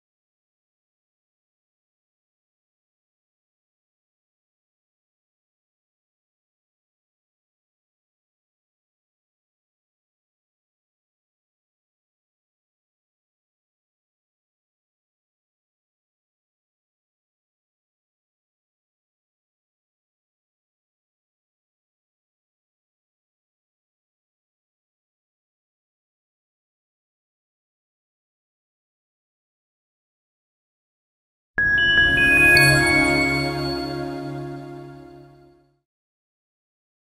Corona Virus Spreading in Blood Animation | 4K | FREE TO USE
This Corona Virus Spreading in Blood Animation | 4K video can use for your any kind of projects, just download video and enjoy! This video has created to make your projects more cool. Best quality and easy to use.